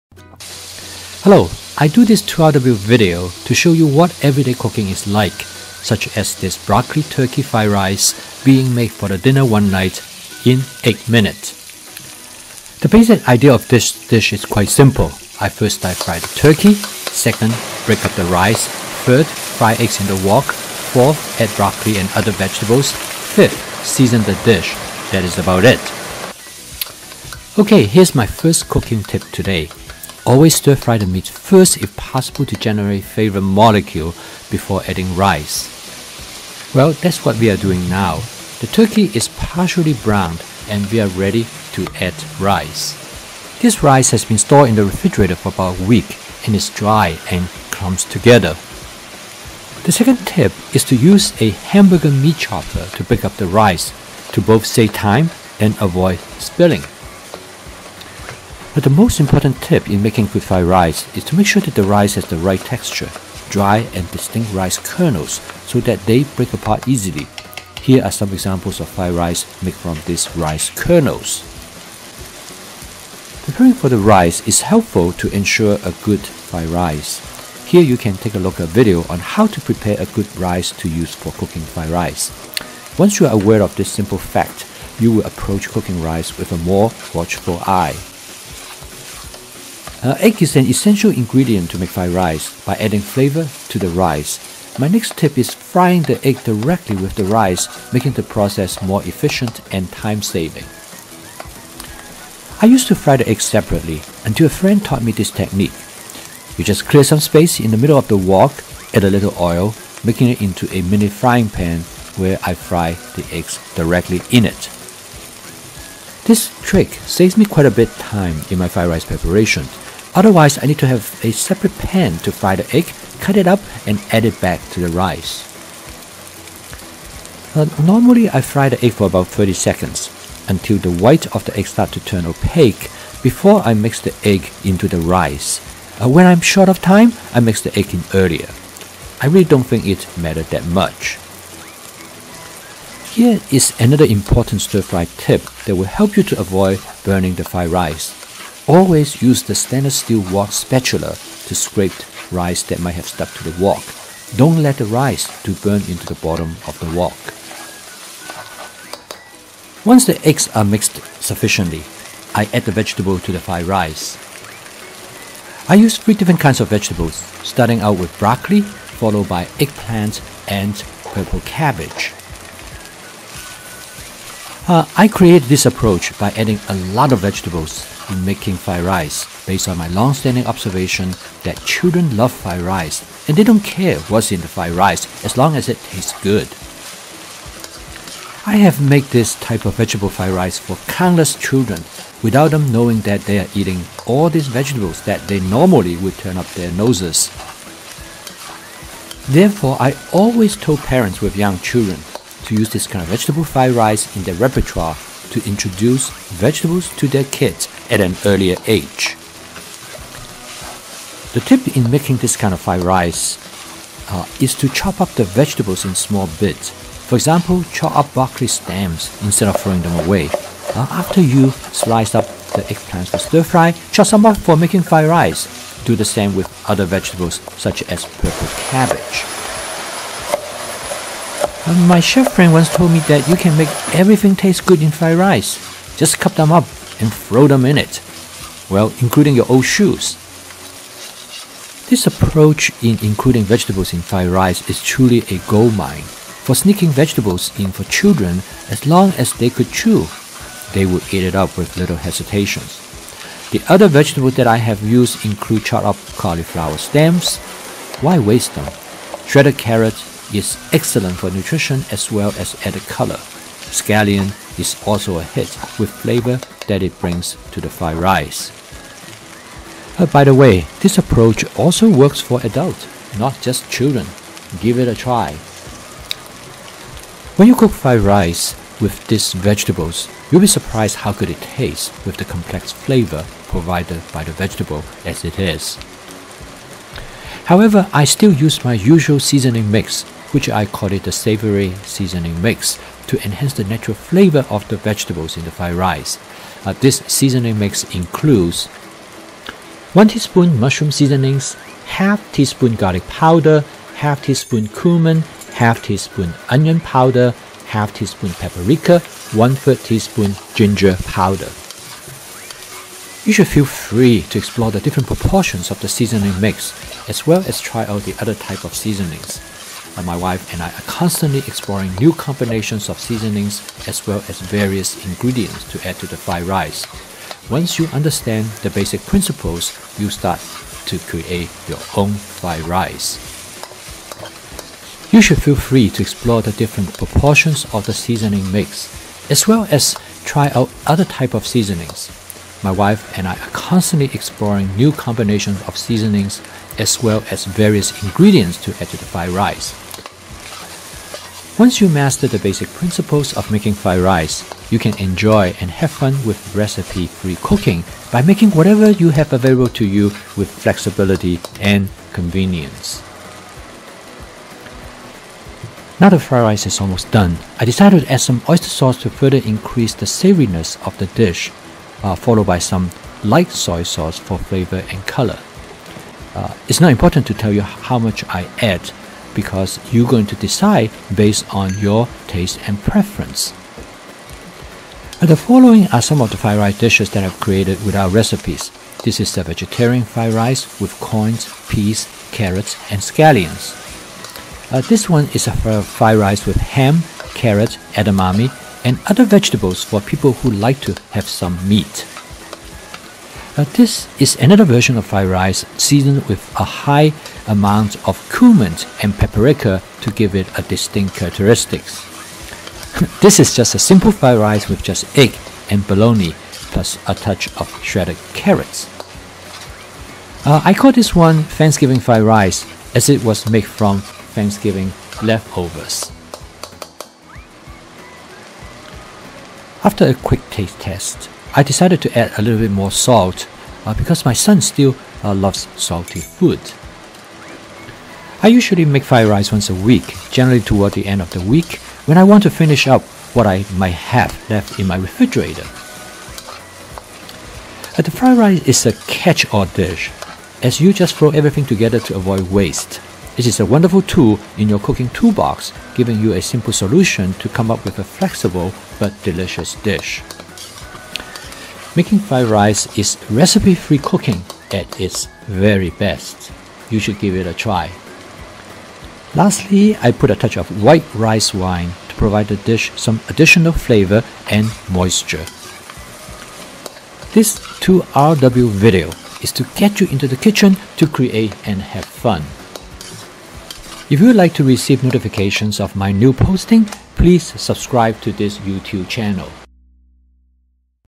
Broccoli Turkey Fried Rice in 8 min. Recipe-free Real-time Wokking
I created this fried rice dish on the fly based on what I found in my refrigerator and freezer.  This dish is an example of my everyday cooking, simple, straightforward, and fun.  I used my basic fried rice principle to cook this dish with no recipe.  I believe that you can do the same.  This fried rice is full of vegetables, making it a great way to introduce vegetables to children, because everybody loves fried rice.  Using this approach, you can make a normally unhealthy dish into a flavorful and nutritious meal at the same time.